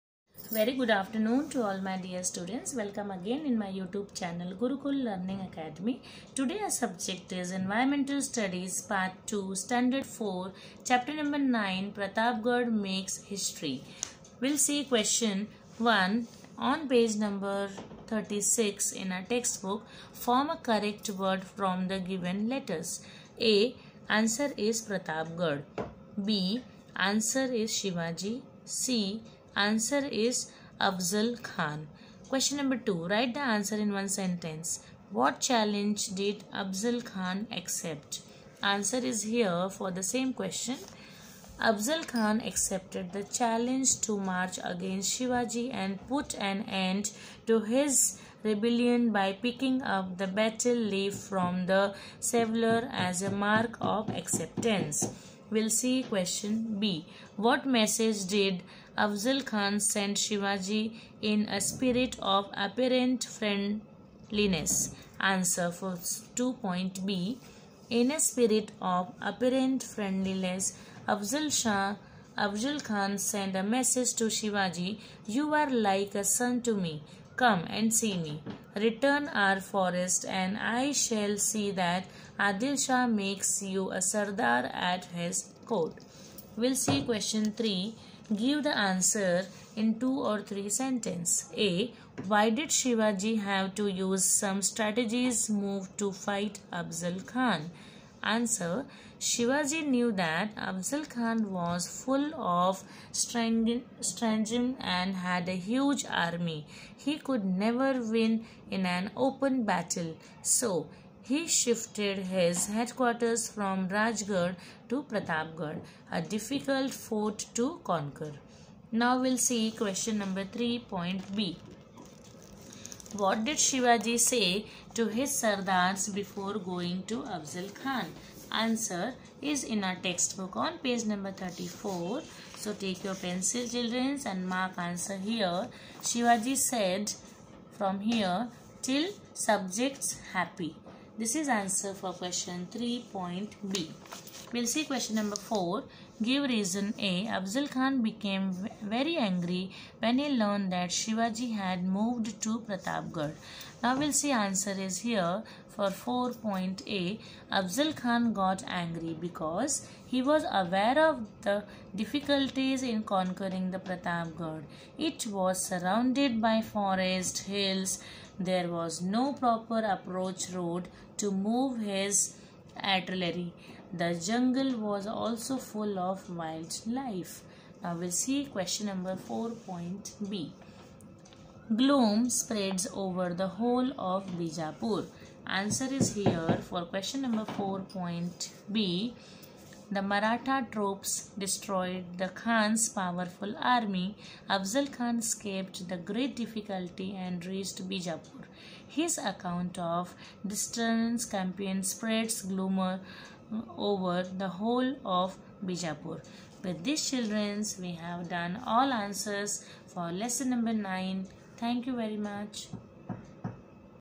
Class 4 - EVS Part 2 lesson 9 Pratapgad makes History Question Answer Maharashtra State Board
Class 4 - EVS Part 2
lesson 9
Pratapgad makes History
Question Answer